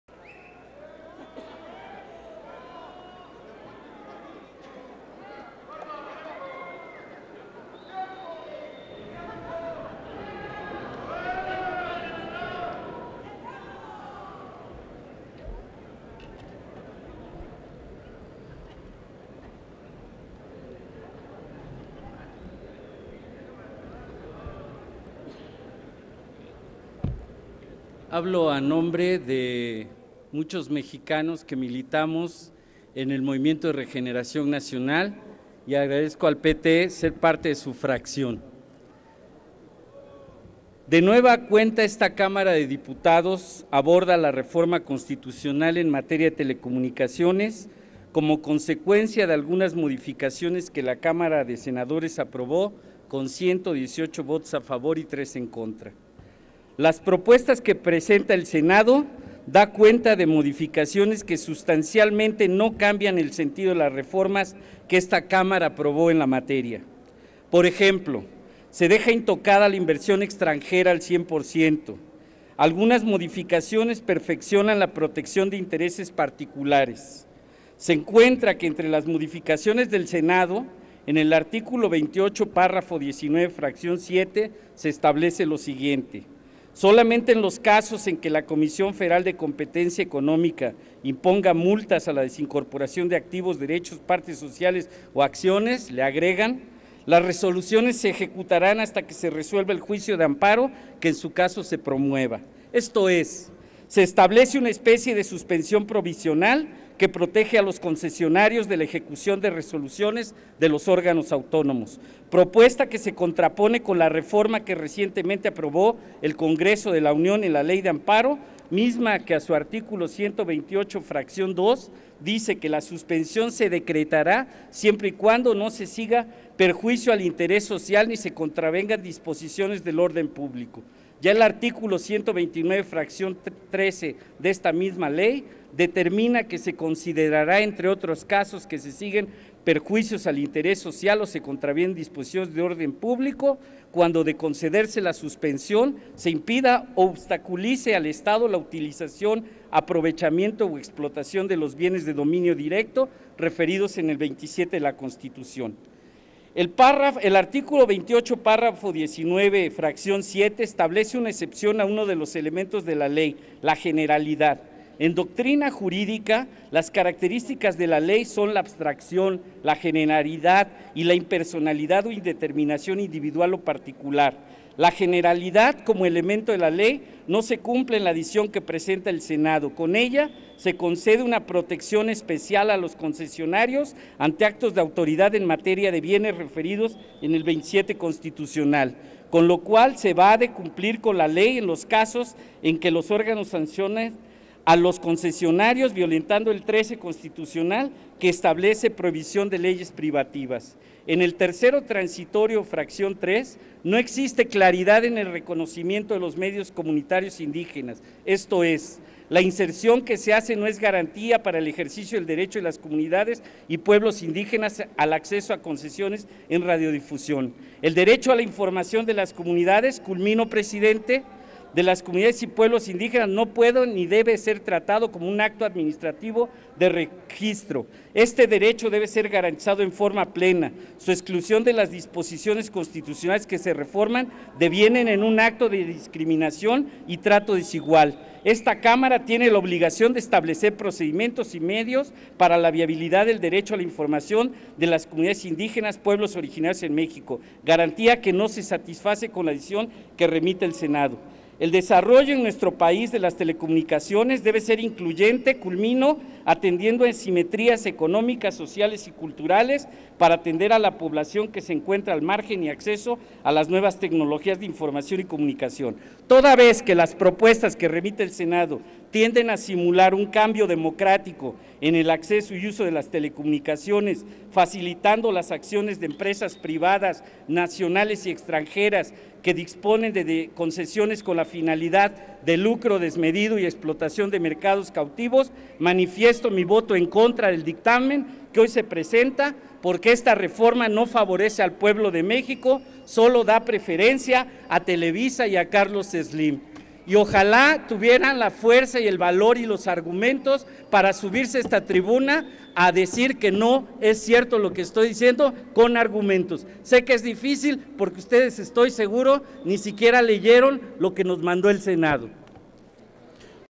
PT en contra del dictamen que reforma Ley de Telecomunicaciones por no favorecer al pueblo Mexicano.
Diversas disposiciones constitucionales en materia de telecomunicaciones.
Sesión Ordinaria del 25 de Abril 2013